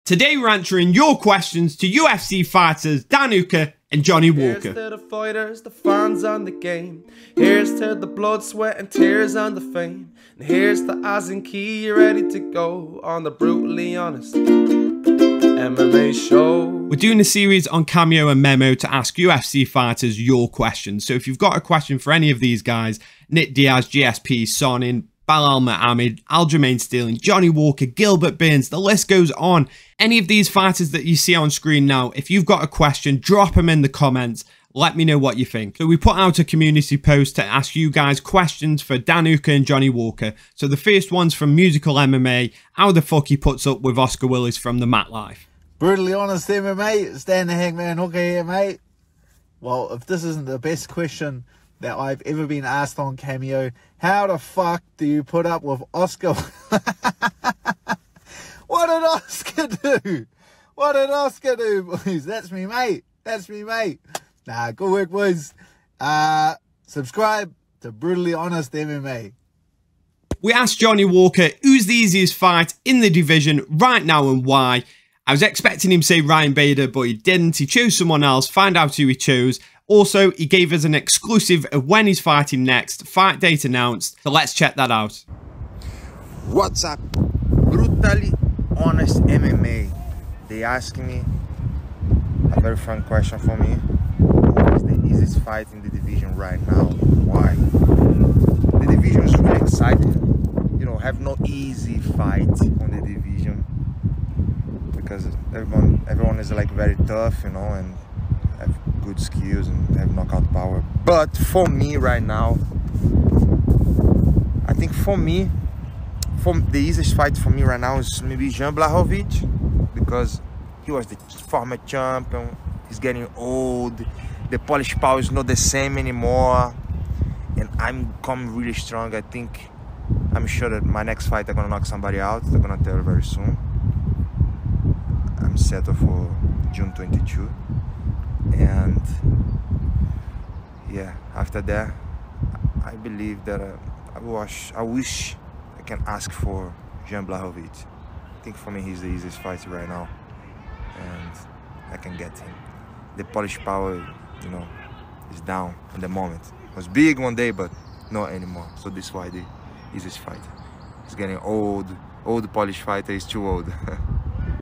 Johnny Walker Calls Jan Błachowicz Easiest Fight in The Division
We put your questions to UFC fighters Johnny Walker and Dan Hooker. Want to ask a UFC fighter a question drop it in the comments and see if it makes the next video

Timestamps
00:00 Intro
00:16 Ask A Question?
00:40 MusicalMMA's Question
00:45 Dan Hooker
01:10 Question?
01:25 Johnny Walker